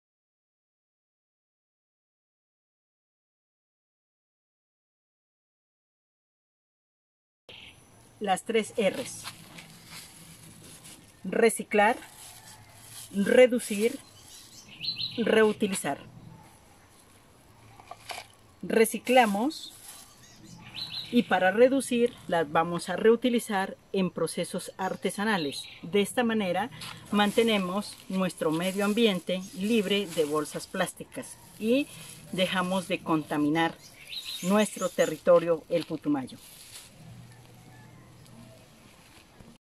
CULTURA PARTE DE LA SOSTENIBILIDAD AMBIENTAL
Las 3R de la ecología: Reducir, Reutilizar y Reciclar. Las tres erres (3R) es una regla para cuidar el medio ambiente, específicamente para reducir el volumen de residuos o basura generada. En pocas palabras, las 3R ayudan a tirar menos basura, a ahorrar y a ser un consumidor más responsable.